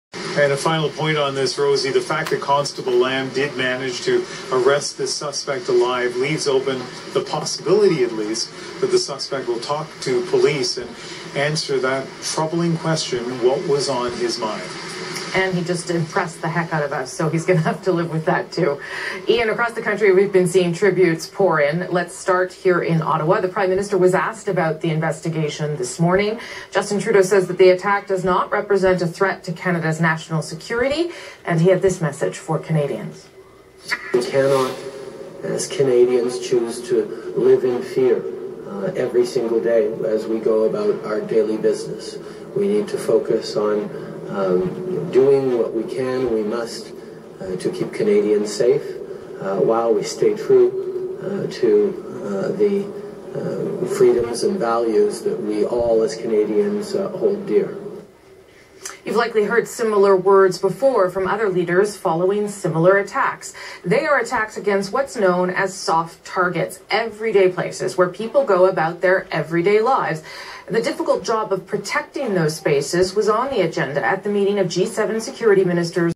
Const LAM arrested him alive - Prime Minister Justin Trudeau's msg 0116 20180424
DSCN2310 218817